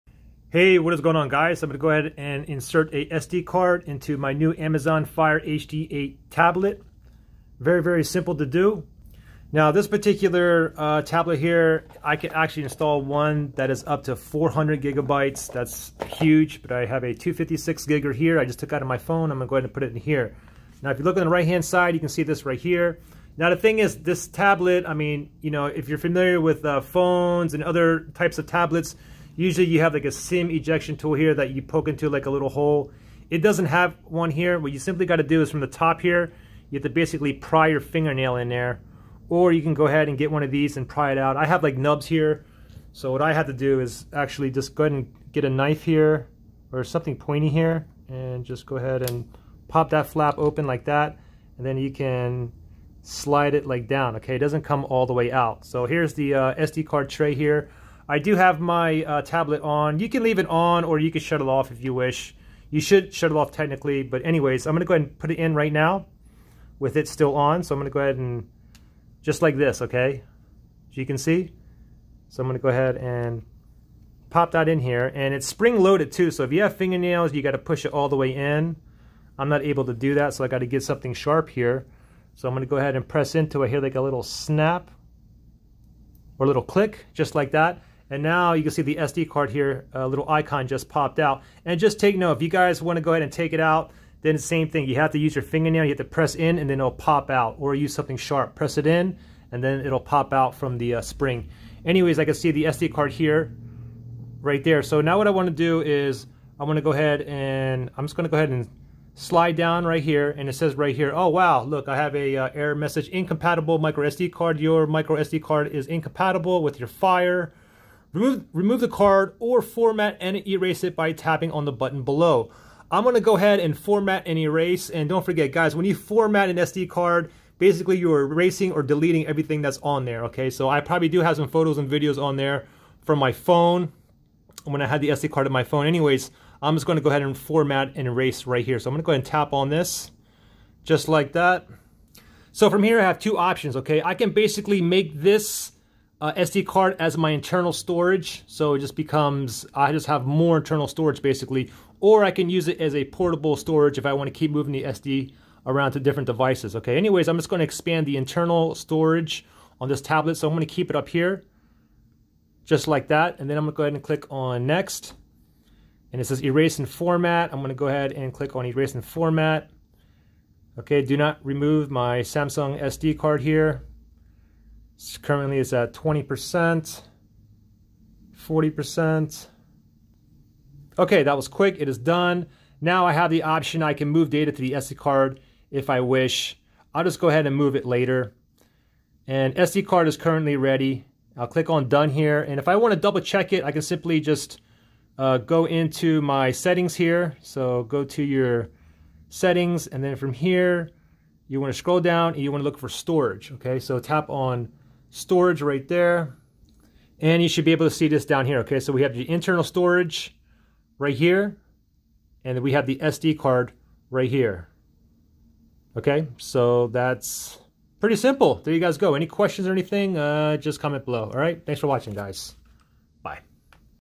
Amazon Fire HD 8 Tablet: How to Insert / Eject SD Card Properly & Check
I show you how to put in and take out the micro sd card on the Amazon Fire HD 8 Tablet. Note that the largest size card you can insert is a huge 400GB. Hope this helps.

Fire HD 8 Tablet (8" HD Display)